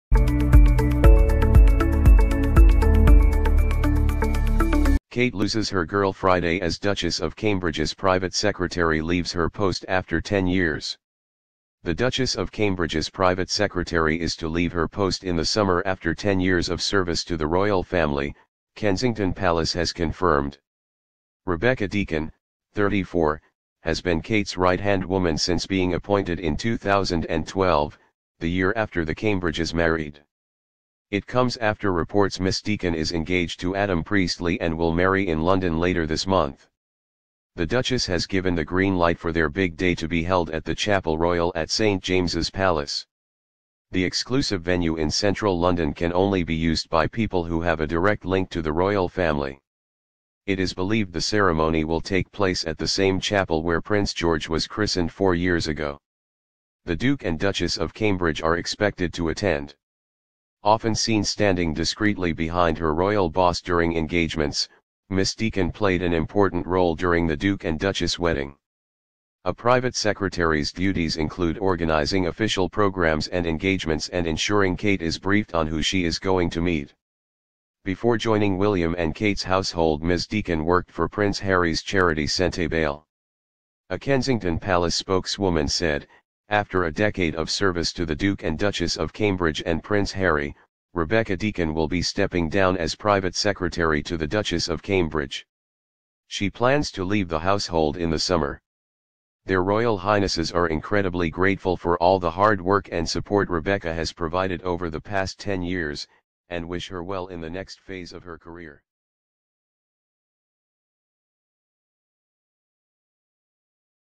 Kate loses her Girl Friday as Duchess of Cambridge's
Kate loses her Girl Friday as Duchess of Cambridge's private secretary leaves her post after 10 years
The Duchess of Cambridge's private secretary is to leave her post in the summer after 10 years of service to the royal family, Kensington Palace has confirmed.
Rebecca Deacon, 34, has been Kate's right-hand woman since being appointed in 2012, the year after the Cambridges married.
It comes after reports Miss Deacon is engaged to Adam Priestley and will marry in London later this month. The Duchess has given the green light for their big day to be held at the Chapel Royal at St James's Palace.
The exclusive venue in central London can only be used by people who have a direct link to the Royal Family.
It is believed the ceremony will take place at the same chapel where Prince George was christened four years ago. The Duke and Duchess of Cambridge are expected to attend.
Often seen standing discreetly behind her royal boss during engagements, Miss Deacon played an important role during the Duke and Duchess' wedding. A private secretary's duties include organising official programmes and engagements and ensuring Kate is briefed on who she is going to meet.
Before joining William and Kate's household Ms Deacon worked for Prince Harry's charity Sentebale. A Kensington Palace spokeswoman said: 'After a decade of service to the Duke and Duchess of Cambridge and Prince Harry, Rebecca Deacon will be stepping down as private secretary to the Duchess of Cambridge.
'She plans to leave the household in the summer.
'Their Royal Highnesses are incredibly grateful for all the hard work and support Rebecca has provided over the past ten years, and wish her well in the next phase of her career.'